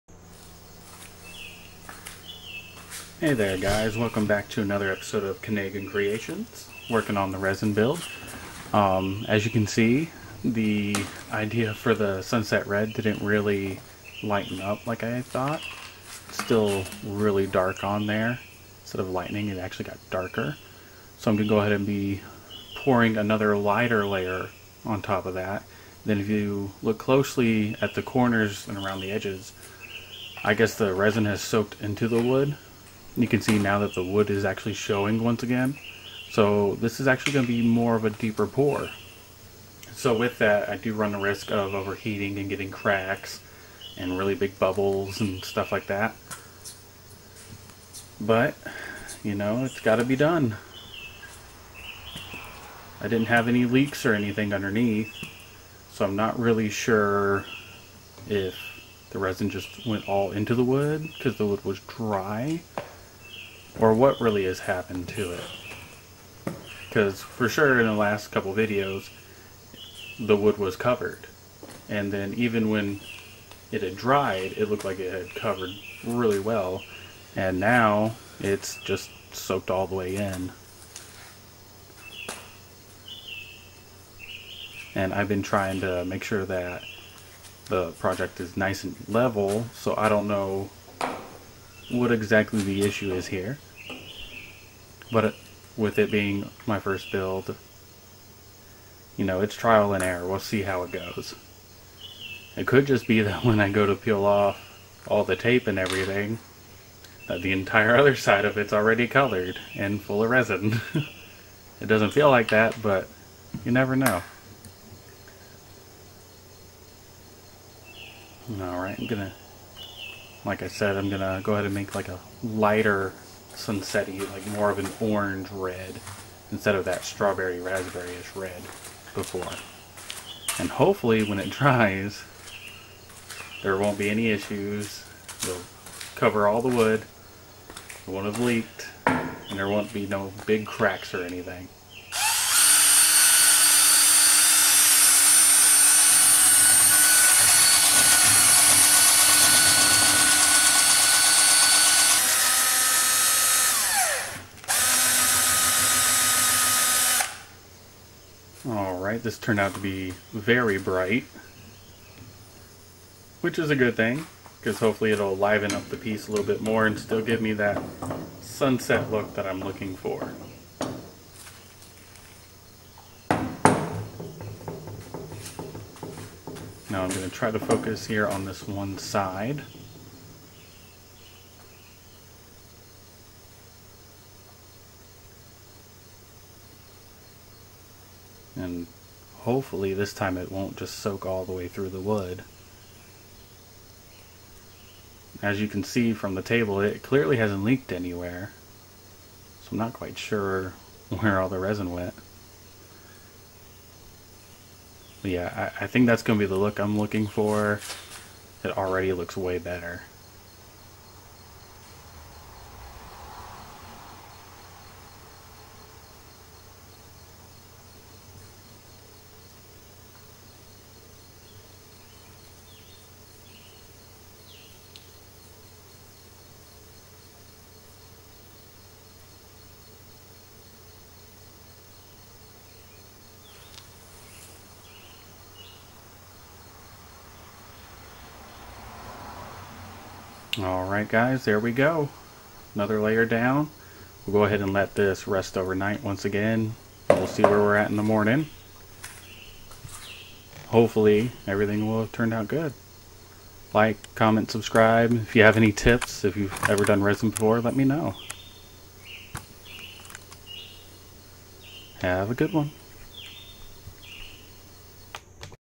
Resin Project Base Work Continued
Hey guys today we're gonna continue working on the base of the project. So far it's been going good and I hope that continues!!